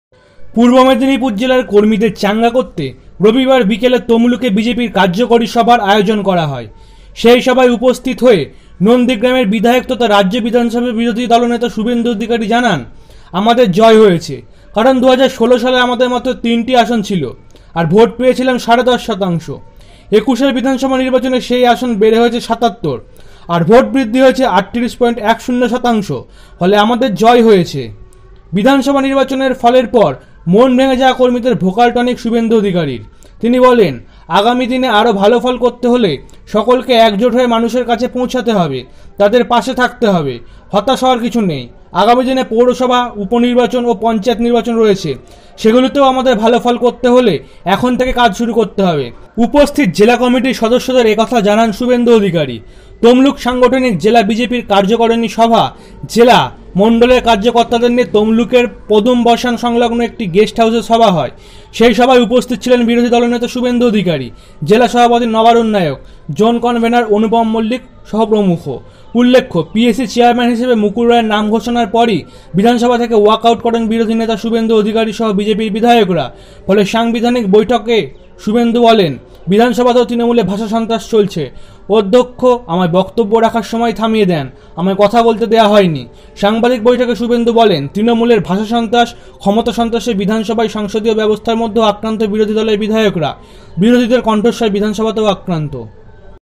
BREAKING- পশ্চিমবঙ্গ বিধানসভা ভোটে জয়ী হয়েছে বিজেপি। ভয়ংকর সত্য ফাঁস করলেন শুভেন্দু।হইচই পড়ে গেল
westbengalelection2021exit polls
bidhansava election result 2021